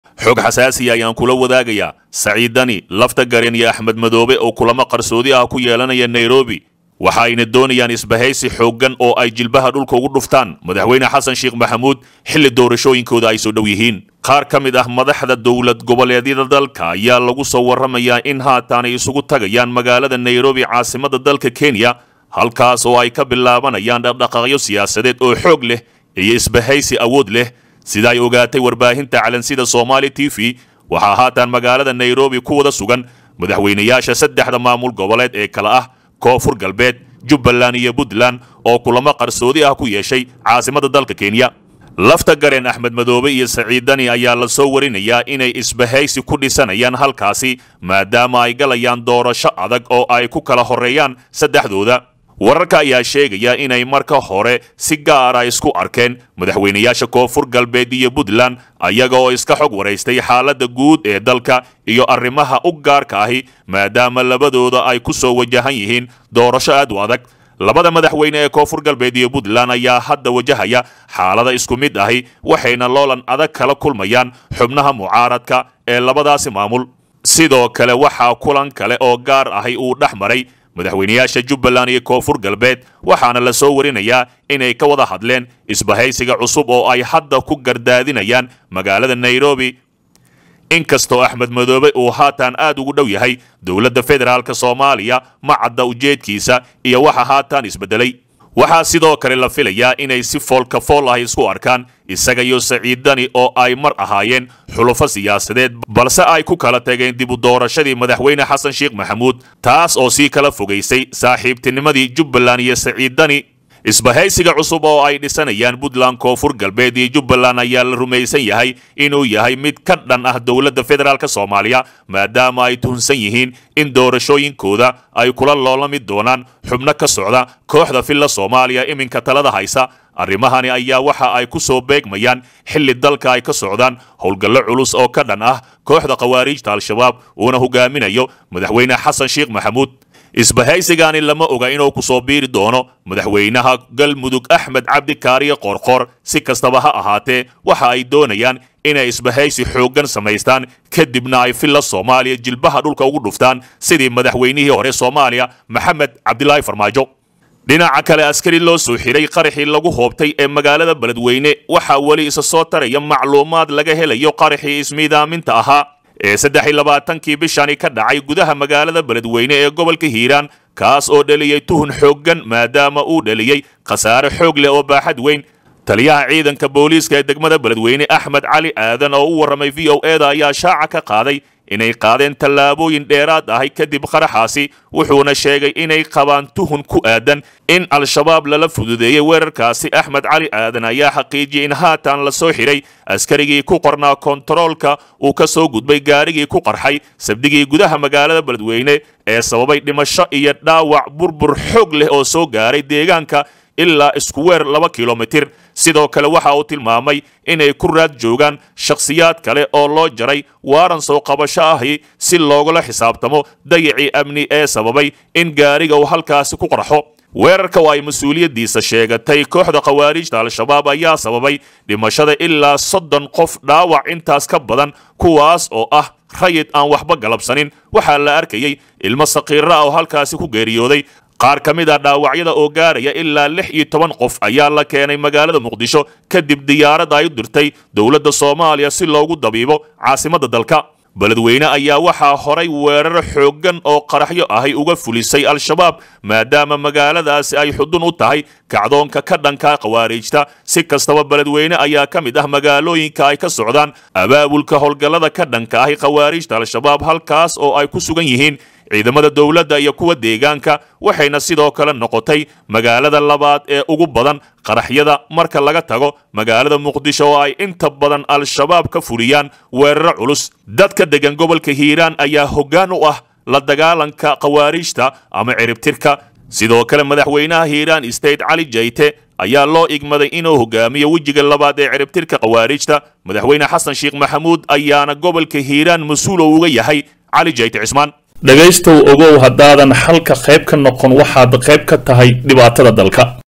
DEG DEG Deni & Axmed Madoobe oo Hada u midoobay in jilbaha dhulka ugu ugu aasaan Xasan Garguurte is
DEG DEG Deni iyo Axmed Madoobe oo Hada u midoobay in jilbaha dhulka ugu ugu aasaan Xasan Garguurte is Baheysi iyo mucaarad cusub xog weyn Dagaal Siyaasadeed Koow